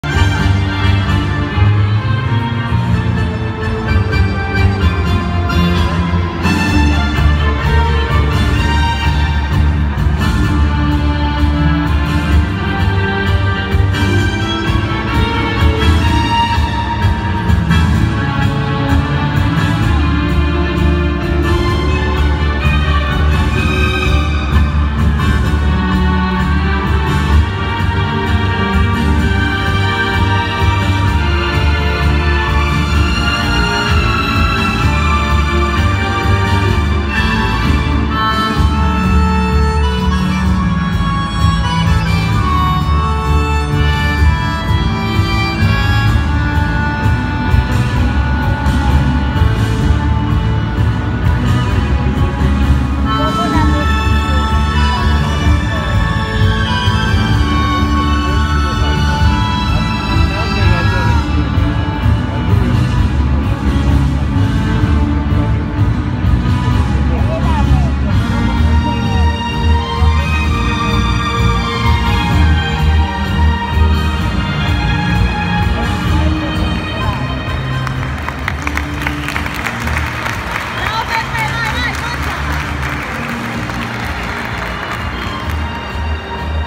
Campionato Regionale FIDS Campania 2021| Senior 2 “B1” Tango | Giuseppe Ruocco & Valeria Savoia
Sito web: asd-italdance.business.site/?m=true
Segui ITALDANCE su Odysee. Link qui

Sostieni il nostro canale con i tuoi LIKE, batti con le mani per apprezzare il nostro lavoro per sostenerci devi registrarsi gratuitamente, segui